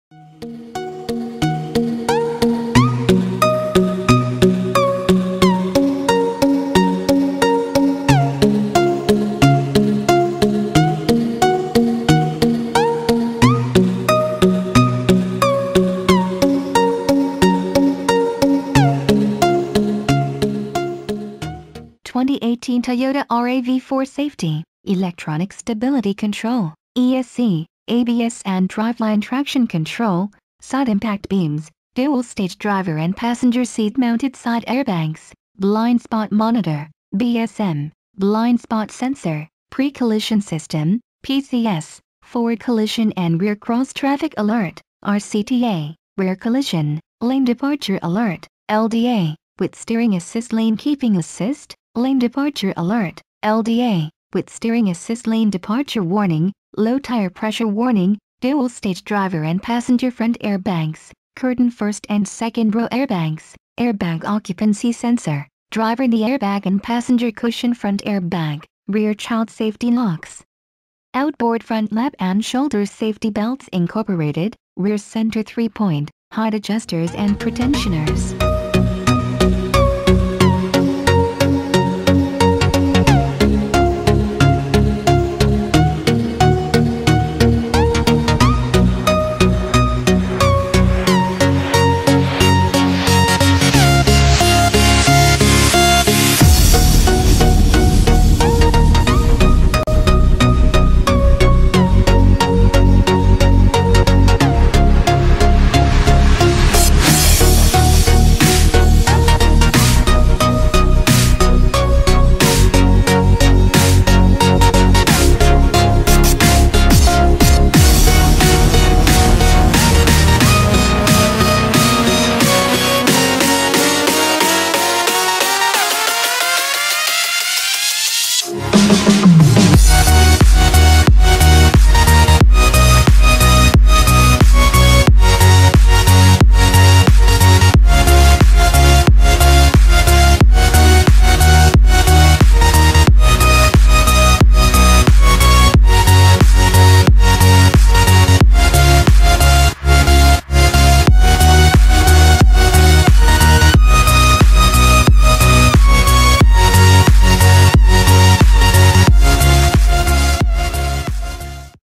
2018 Toyota RAV4 SAFETY
2018 Toyota RAV4 SAFETY
Electronic Stability Control (ESC)
ABS And Driveline Traction Control
Side Impact Beams
Dual Stage Driver And Passenger Seat-Mounted Side Airbags
Blind Spot Monitor (BSM) Blind Spot Sensor
Pre-Collision System (pcs) Forward Collision and Rear Cross-Traffic Alert (rcta) Rear Collision
Lane Departure Alert (lda) w/Steering Assist Lane Keeping Assist
Lane Departure Alert (lda) w/Steering Assist Lane Departure Warning
Dual Stage Driver And Passenger Front Airbags
Curtain 1st And 2nd Row Airbags
Airbag Occupancy Sensor
Driver Knee Airbag and Passenger Cushion Front Airbag
Rear Child Safety Locks
Outboard Front Lap And Shoulder Safety Belts -inc: Rear Center 3 Point, Height Adjusters and Pretensioners